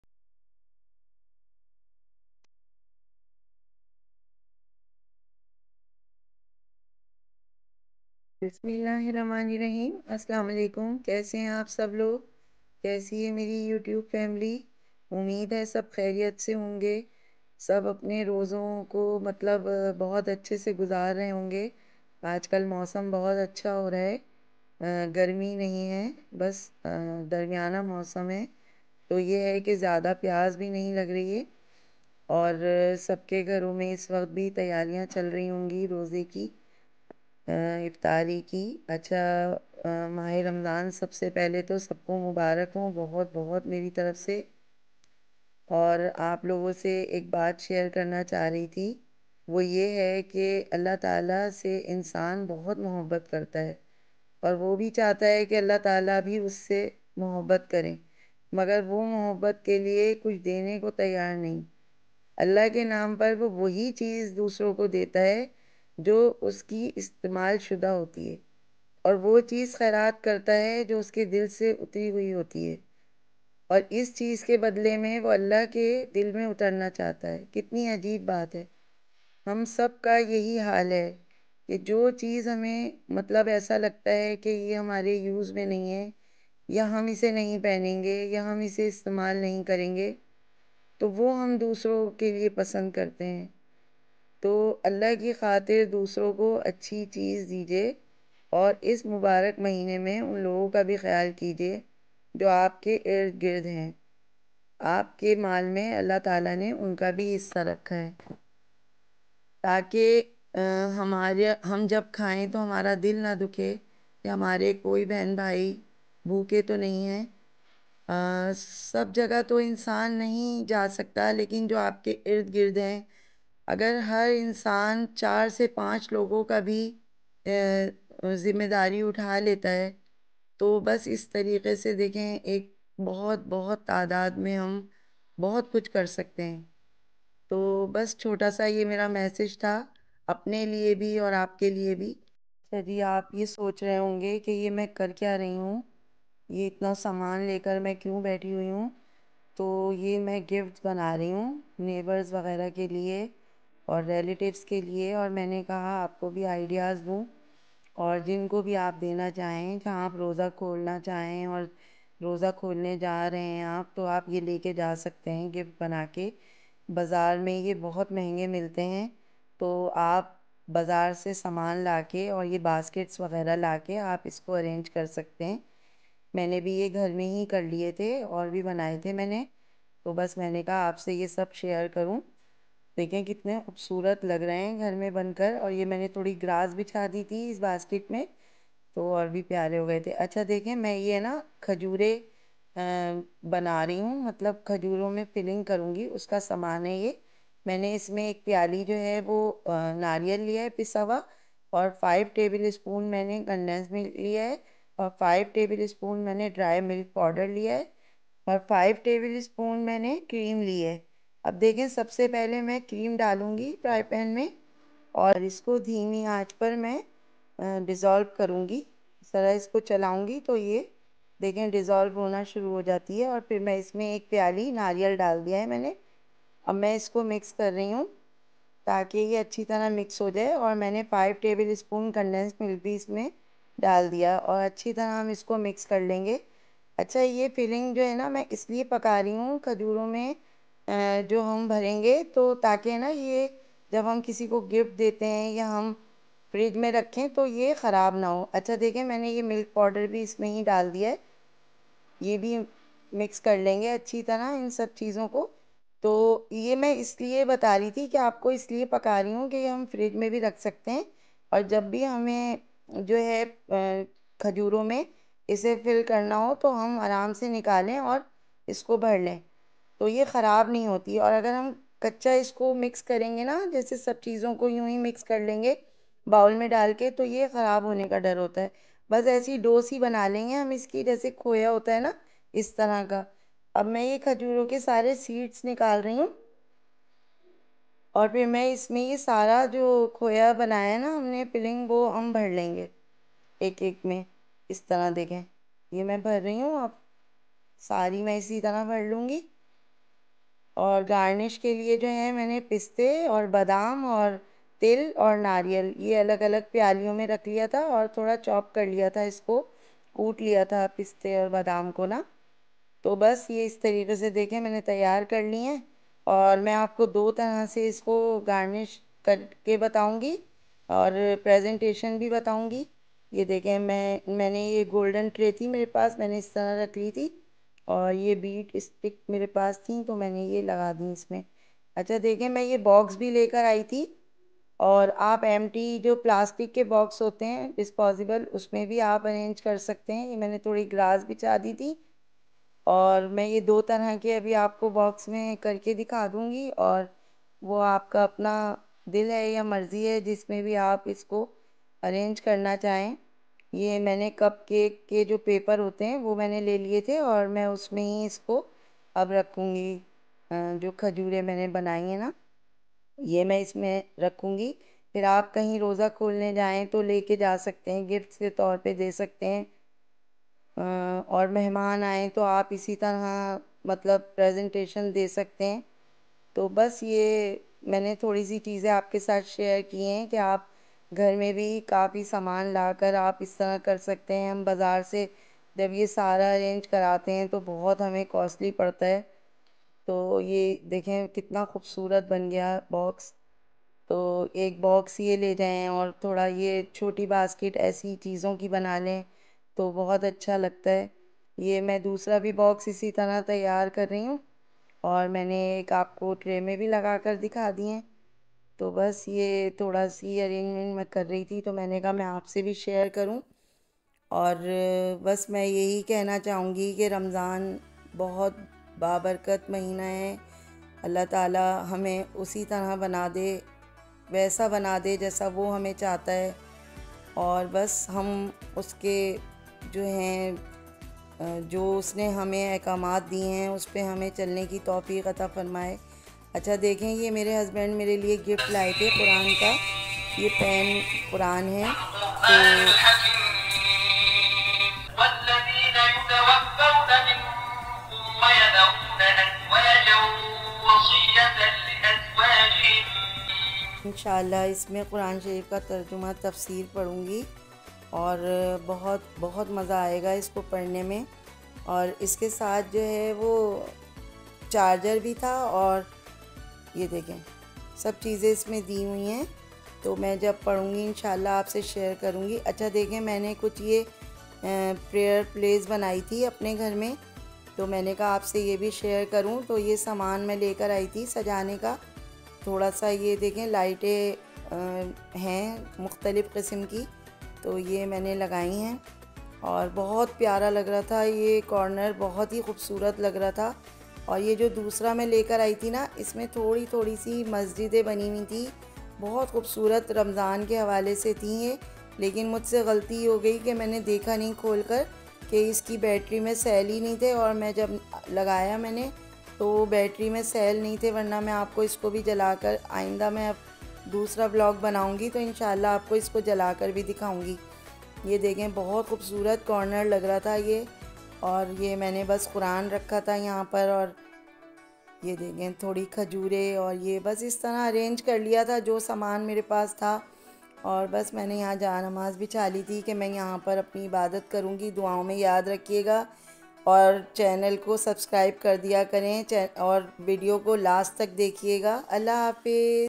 RAMZAN MUBARAK TO MY YOUTUBE FAMILY BY @Foodtuberaliaarshad
Redecorating Pray Area and Ramzan Tehwariyan. Today I am going to show you the best way of making a basket for distributing in Ramzan. And a special recipe for making almond more delicious. Enjoy it and share with others.
Ingredients of filling in dates:
15 Dates
Grinded coconut 10 tbsp
5 tbsp condensed milk
5 tbsp milk powder

For decoration:
Sesame seed
Almonds
Pistachio

"Food Tuber: Elevate Your Taste Experience!"

____________________________/SOCIALLINKS\______________________________